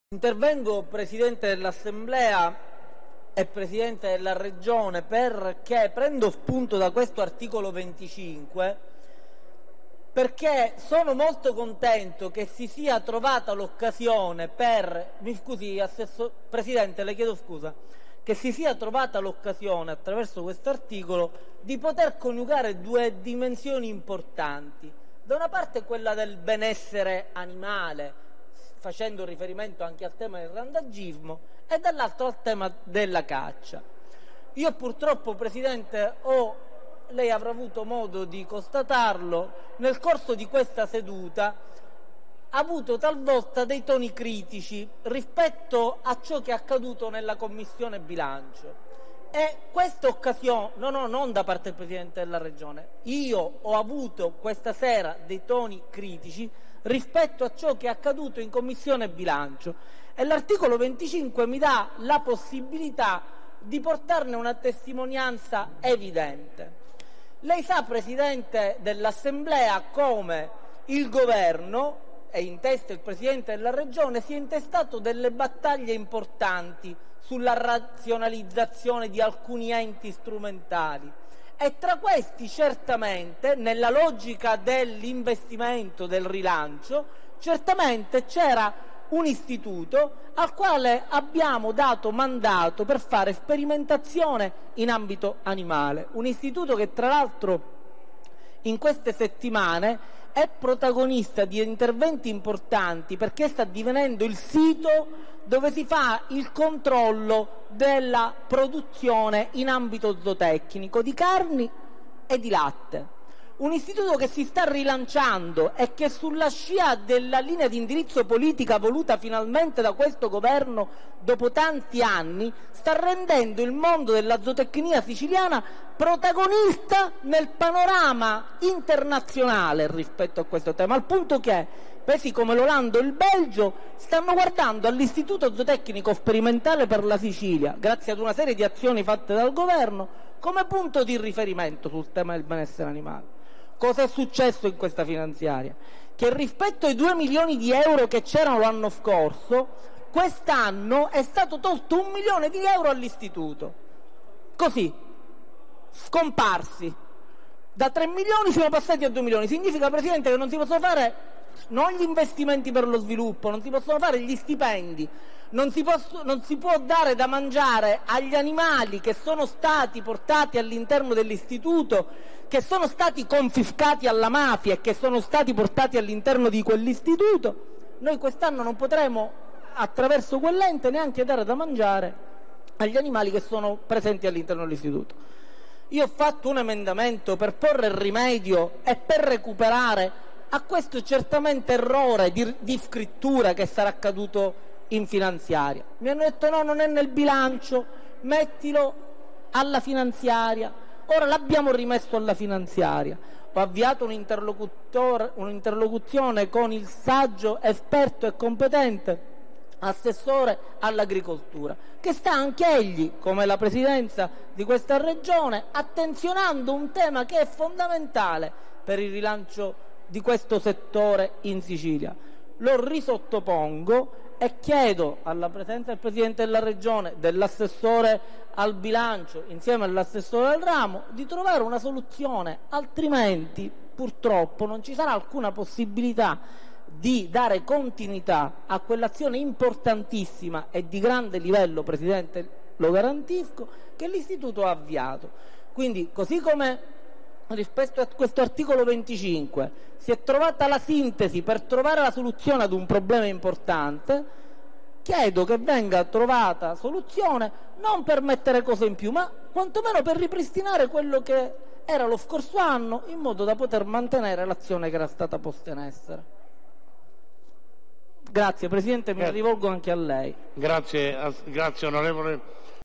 Benessere animale e rilancio della zootecnia siciliana, intervento in Aula
Ars, Finanziaria - Intervento In Aula di Giorno 17 marzo 2021. Benessereanimale e rilancio della zootecnia siciliana.